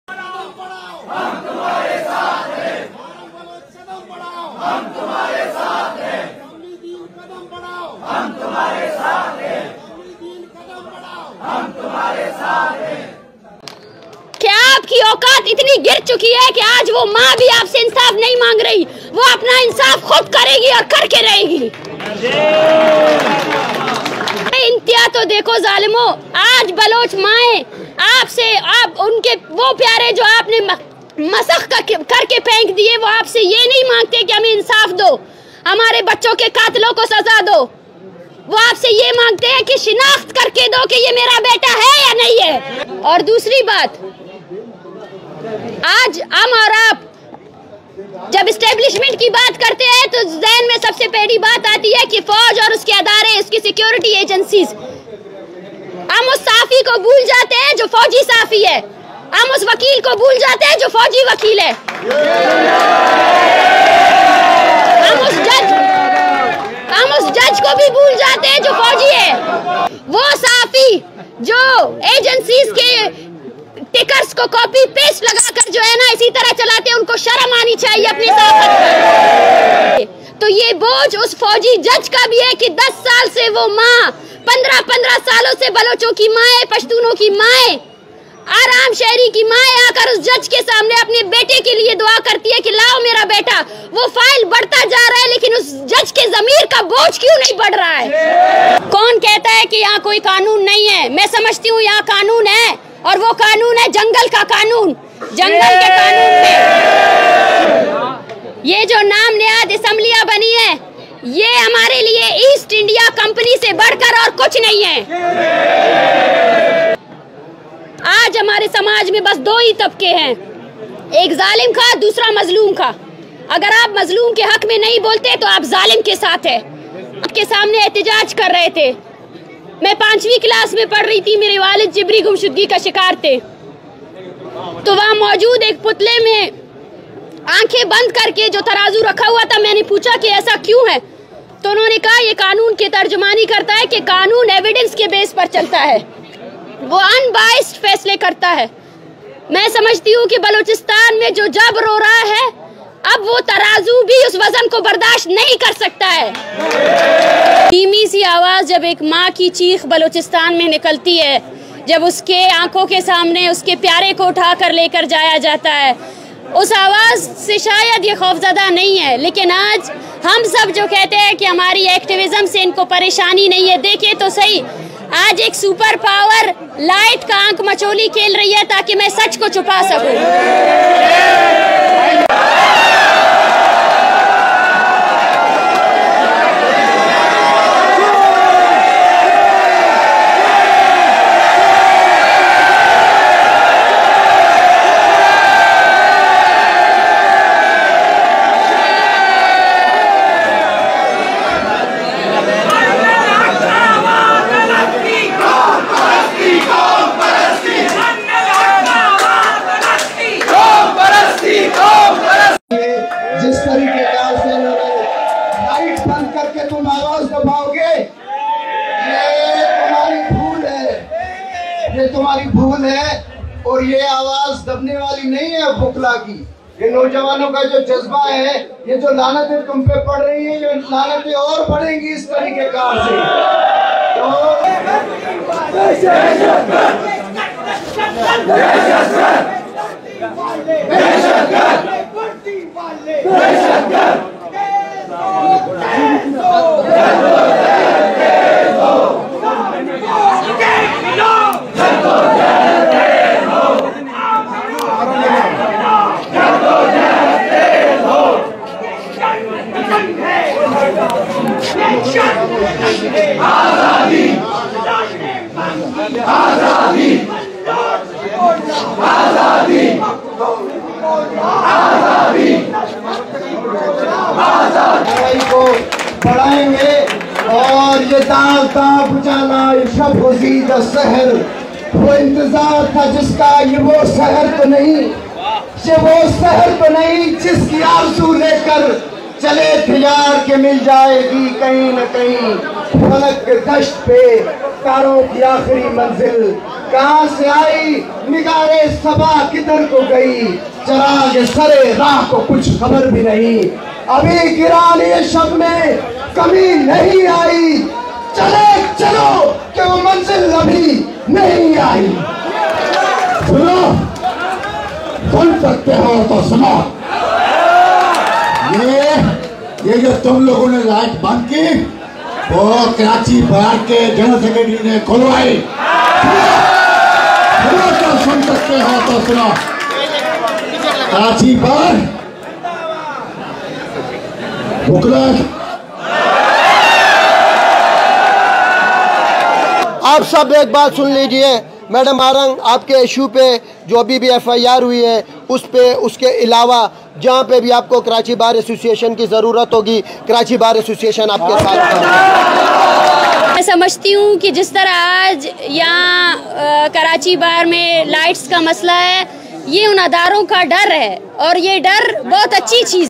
Dr Mahrang Baloch Powerful Speech at Karachi Bar October 16, 2024
Video: Shahantv